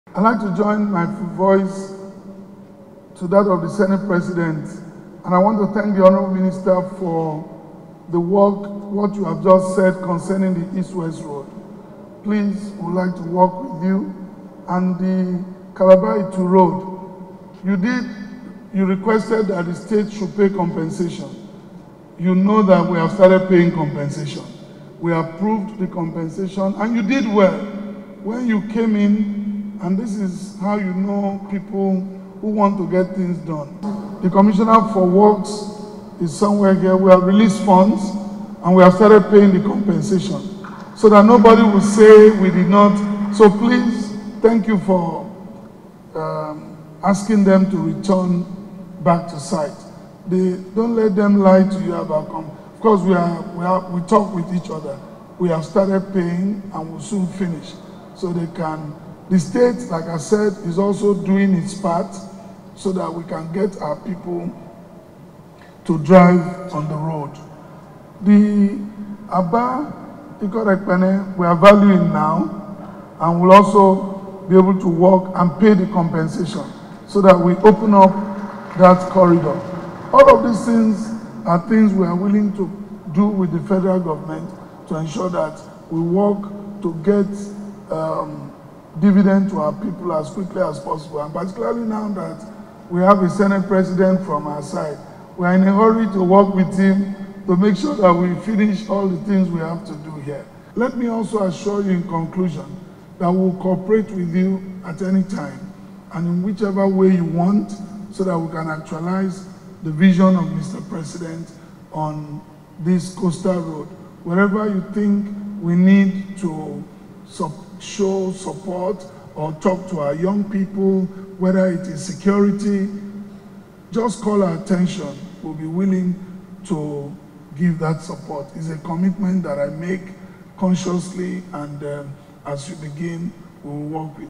WE WILL SUPPORT FEDERAL MINISTRY OF WORKS FOR ROAD INFRASTRUCTURE - PASTOR UMO ENO
Akwa Ibom State Governor, Pastor Umo Eno pledges support of his administration to Federal Ministry Of Works towards road infrastructure.

Pastor Umo Eno gave the assurance during a Stakeholders Meeting on the Lagos-Calabar Coastal Highway held in Uyo.